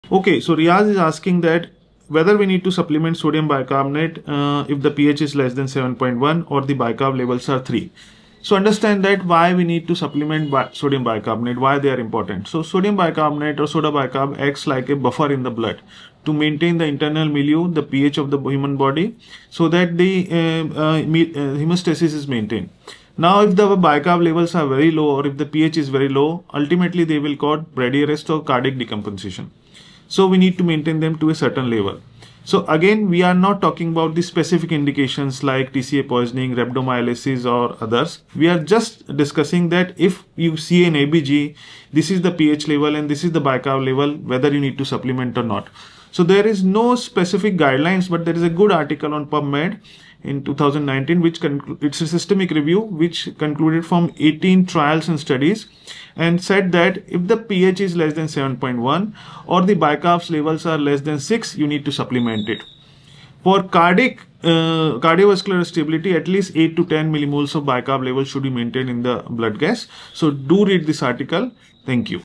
icushort 349: When to supplement sodium bicarbonate based on pH and Hco3 level
The article name is Bicarbonate Therapy for Critically Ill Patients with Metabolic Acidosis: A Systematic Review

When to supplement sodium bicarbonate based on pH and Hco3 level